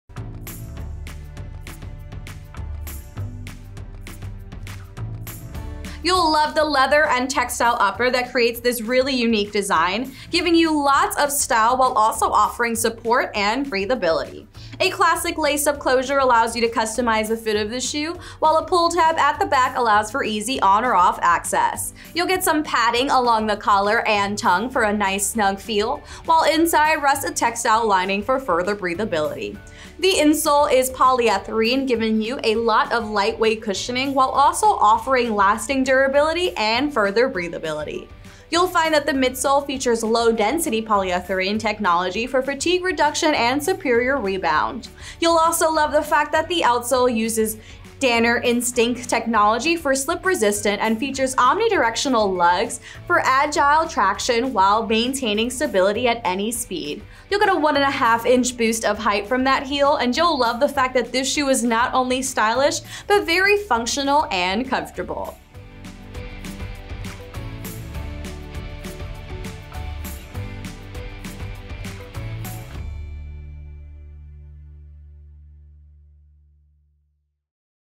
Danner Trail 2650 Campo 3" SKU: 9585474
Leather sourced from a tannery that achieved a Gold Rating from the Leather Working Group (LWG).
Go for an adventure trek donning the super comfy and stylish Danner&reg; Trail 2650 Campo 3" shoes.
Leather and textile upper construction.
Breathable textile lining and insole.
Raised round toe silhouette with protective toe cap.
Removable insole.
Pull tab at the heel.
Padded collar and tongue.
Lace-up closure.
PerfX&trade; polyurethane footbed offers lightweight cushioning, lasting durability, and breathability.
Boundlite midsole features low-density polyurethane technology for fatigue reduction and superior rebound.
Danner Instinct outsole is slip resistant and features omnidirectional lugs for agile traction while maintaining stability at any speed.
Synthetic outsole.
Imported.